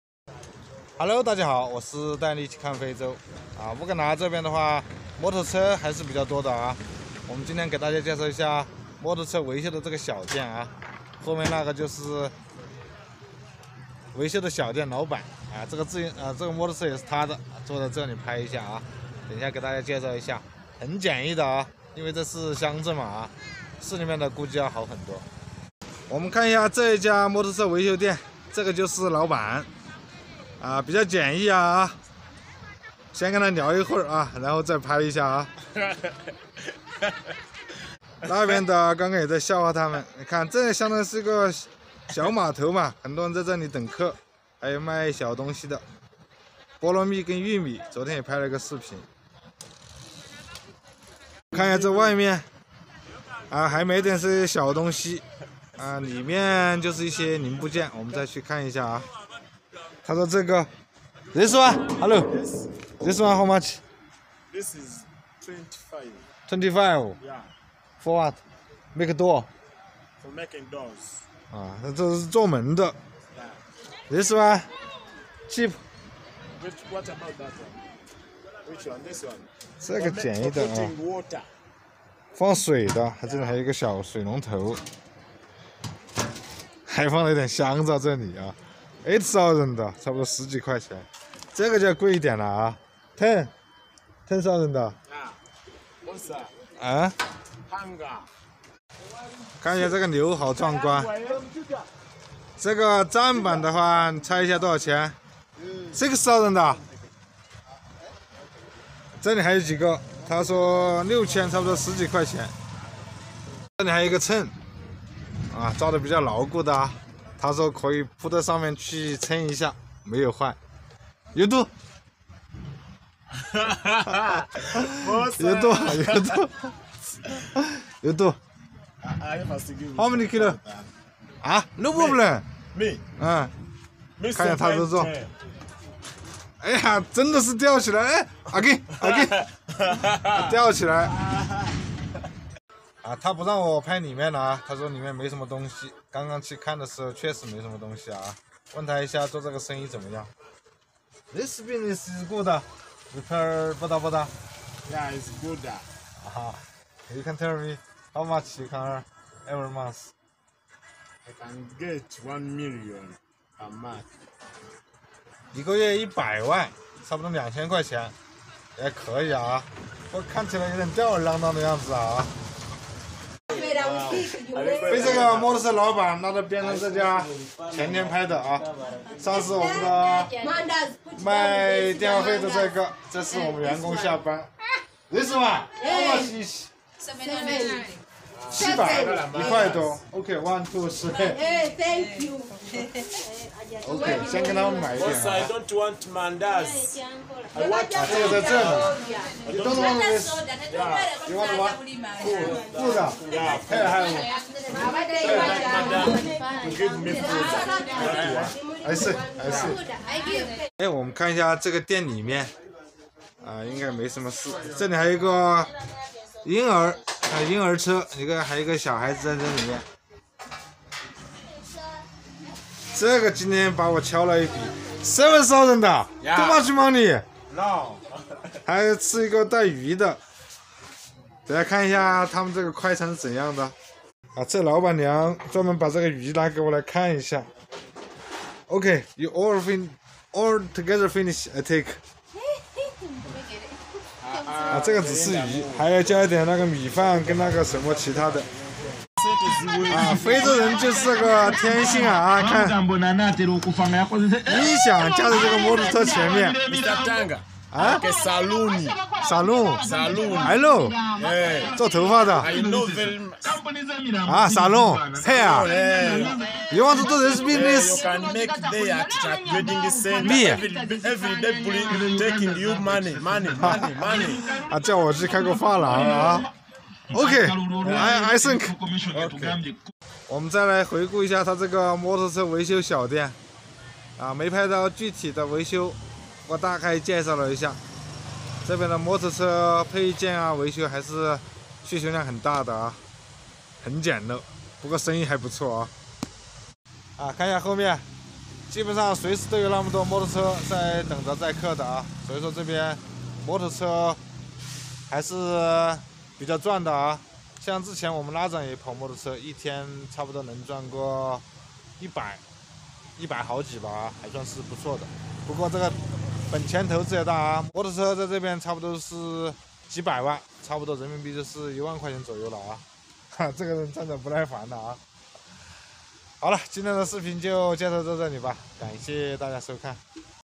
当地摩托车市场庞大，这样的维修小店虽然简陋，但生意却很好！【带你一起看非洲】
这一期是以前的视频，分享到YouTube上也是让大家观看一下最开始的一个情况。
当地摩托车市场庞大，这样的维修小店虽然简陋，但生意却很好！